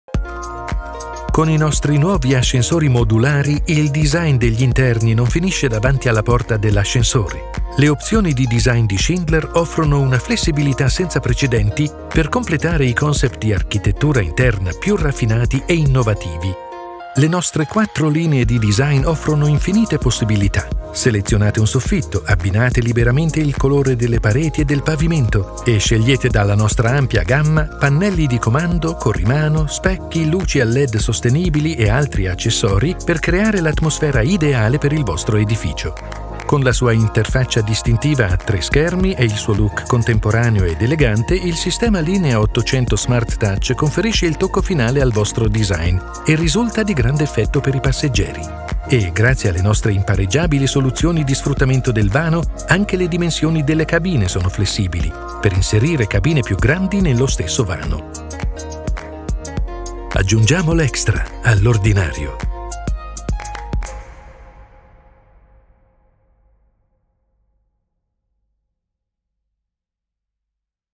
EXTRA DESIGN (italiano)
Scoprite le infinite possibilità di design e i pregiati materiali degli equipaggiamenti – disponibili per l’intera gamma di modelli.